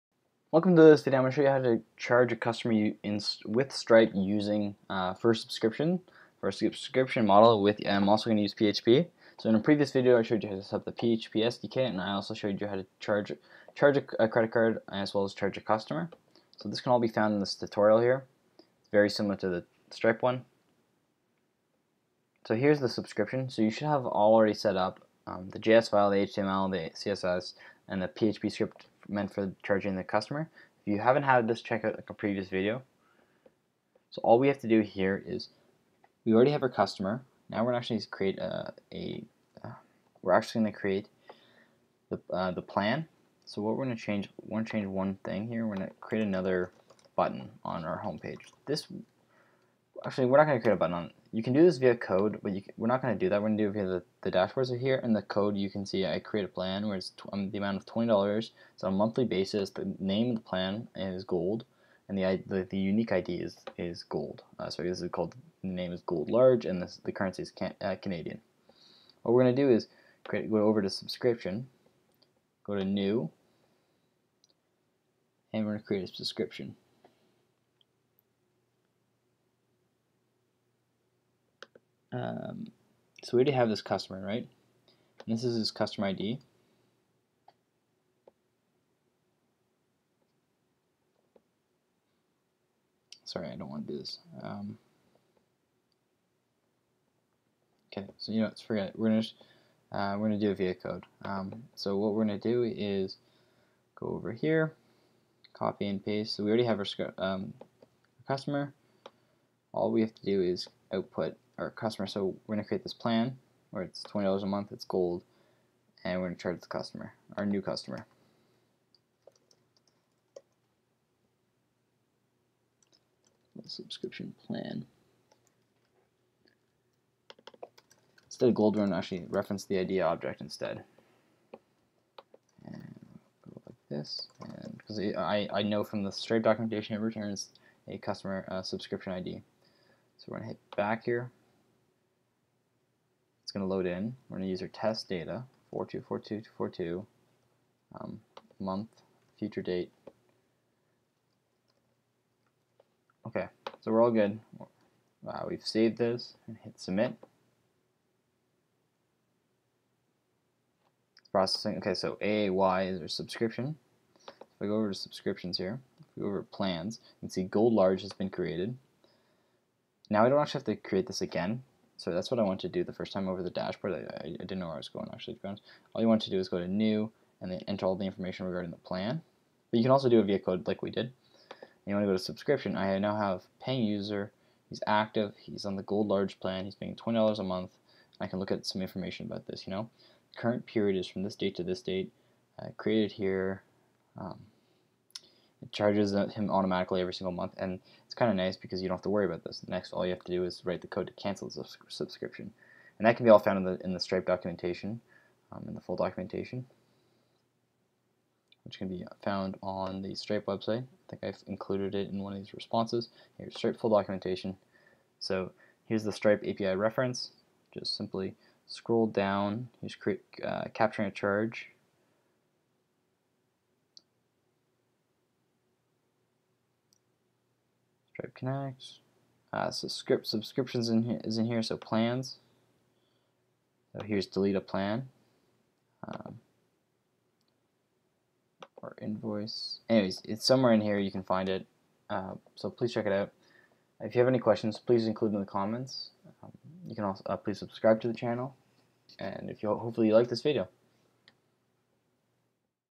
Charging Customer for a Subscription Service using Stripe with PHP
We charge customer (can be second time) using the Stripe payment platform.

We charge with a subscription using the Stripe payment platform.

Equipment:
My mic/headphones